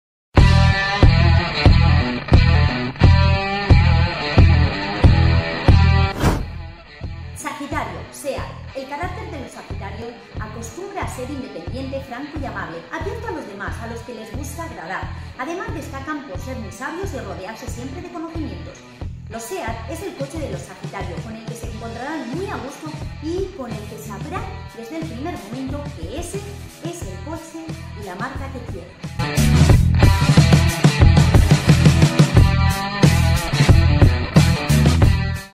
HOROSCOPO ALVATO - SAGITARIO ♐
¿Eres Sagitario? pues tu coche es el SEAT, los sagitarios se caracterizan por ser:

Personas amables, francas e independientes.
Son abiertos a los demás por lo tanto son muy agradables.
Son muy sabios y siempre en busca de conocimiento.

¿Quieres que tu coche quede como recién salido del concesionario? Visita en la web dónde encontrarás la dirección de tu centro más cercano